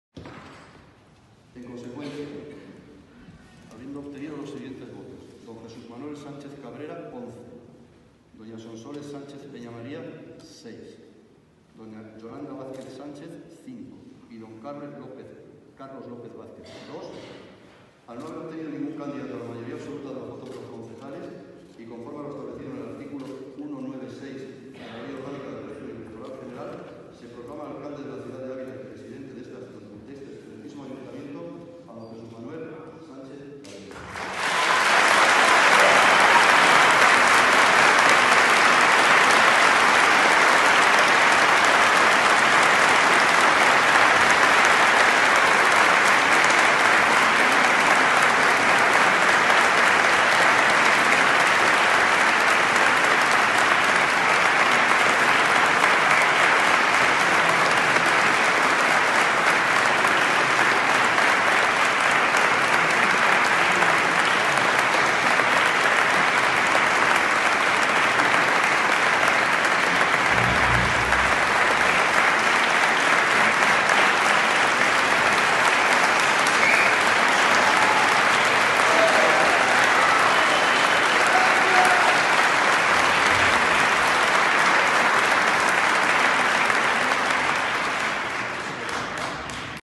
Proclamación de Jesús Manuel Sánchez Cabrera como alcalde de Ávila
Acto de proclamación de Jesús Manuel Sánchez Cabrera, del grupo político Por Ávila, como nuevo alcalde del Ayuntamiento de Ávila, institución en la que este partido tiene representación con once concejales.